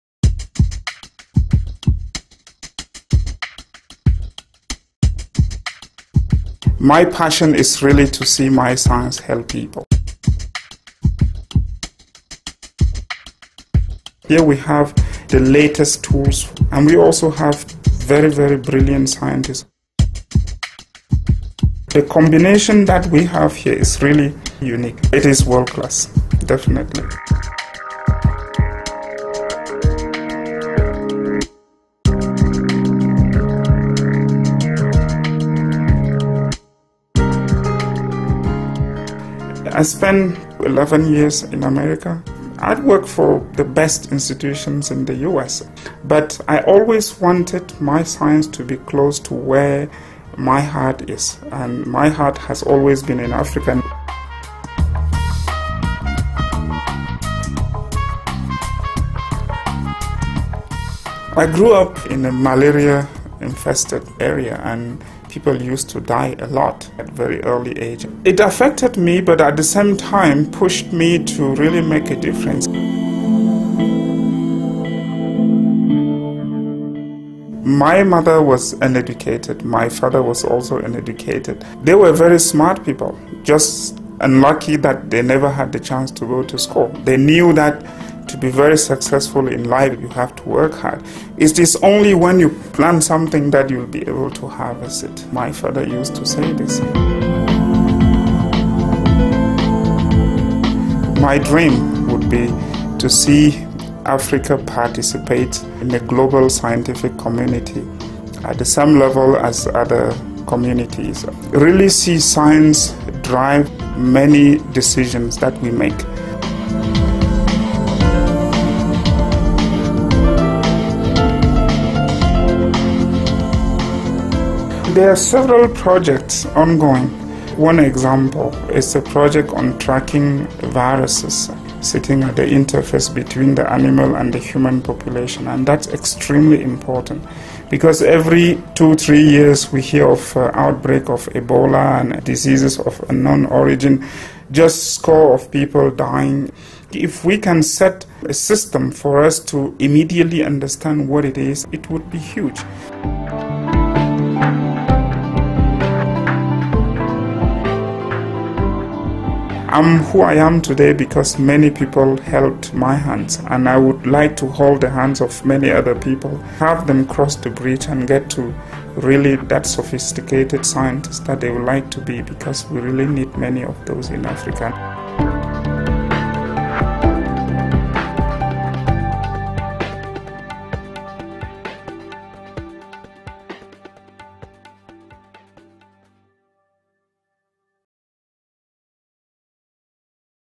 The BecA-ILRI Hub
Research scientist, Appollinaire Djikeng, who manages the Biosciences eastern and central Africa (BecA) Hub at the International Livestock Research Institute (ILRI) speaks of his passion to see science help Africa better participate in the global scientific community and to see more science-driven decision-making in the continent. At the BecA Hub, African scientists are using the latest tools and technologies to find solutions for Africa's most pressing agricultural problems. The photofilm was made in August 2010 for ILRI by duckrabbit, a UK-based multimedia production company.